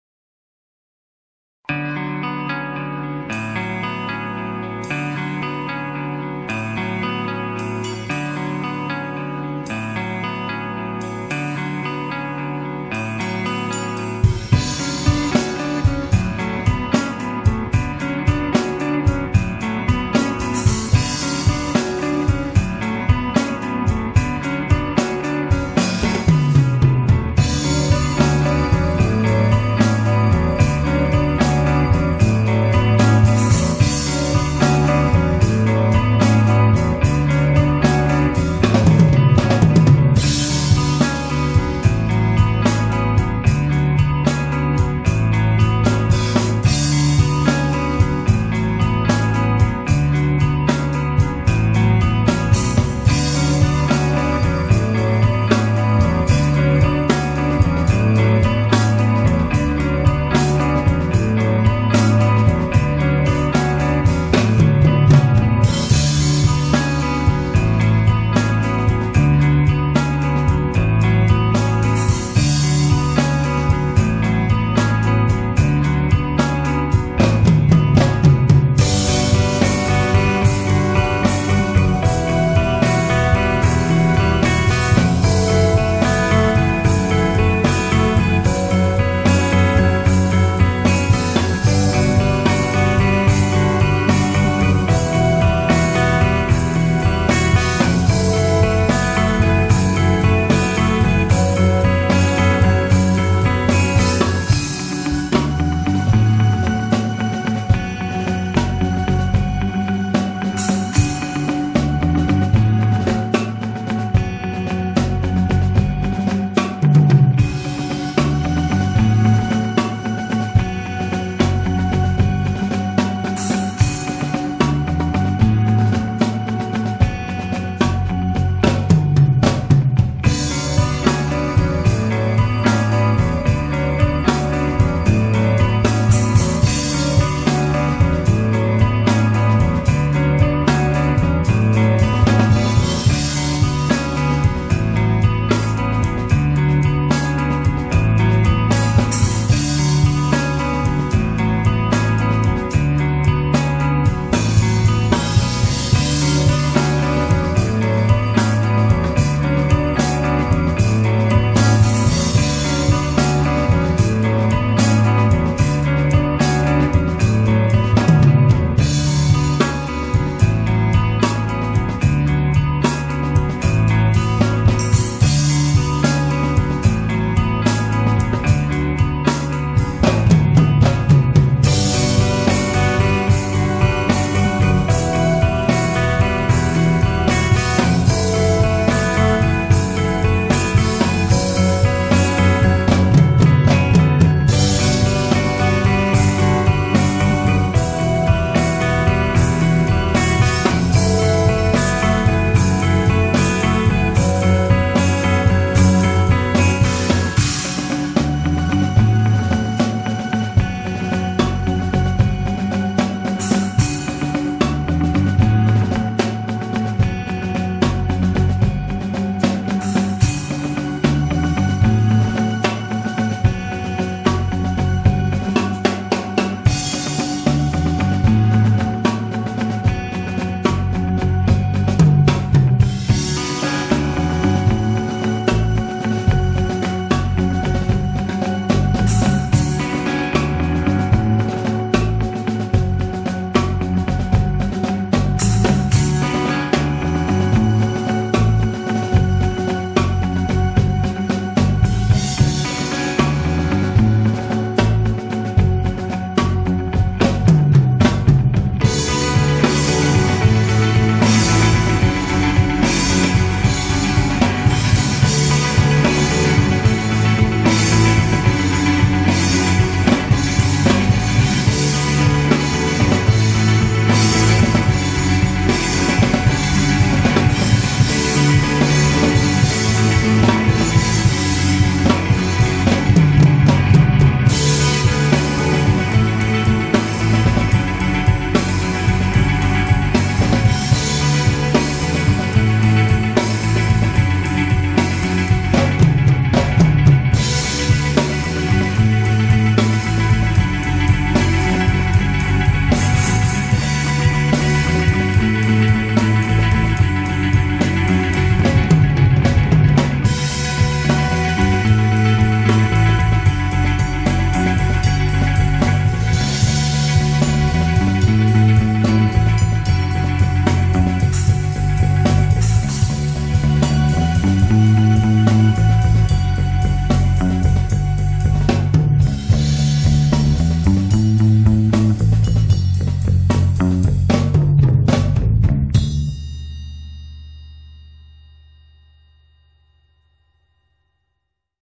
Optimized Audio for Atmospheric Indie Rock Instrumental (Drumless Track)
I remade this video after optimizing the audio. I don't have a DAW I just used Audacity audio editor to boost the bass and treble while also adding compression and a tiny bit of reverb. Here's the original video with non-optimized (raw) audio for comparison.